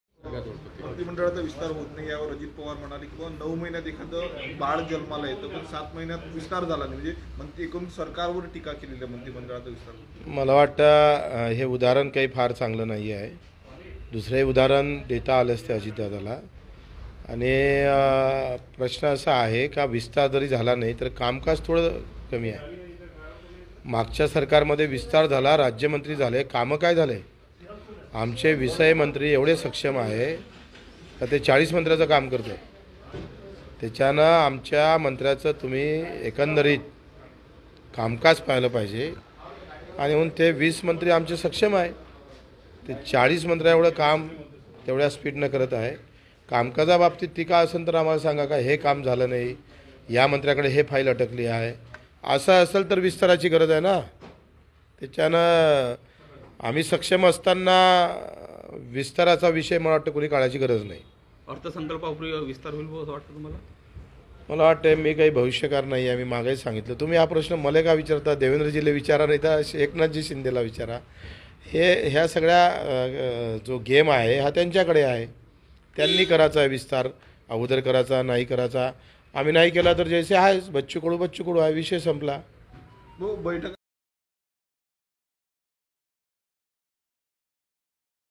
आमचे 20 मंत्री 40 मंत्र्यांच काम करत आहे - आमदार बच्चू कडू | AMRAVATI CITY NEWS | BACCHU KADU
आमचे 20 मंत्री 40 मंत्र्यांच काम करत आहे - आमदार बच्चू कडू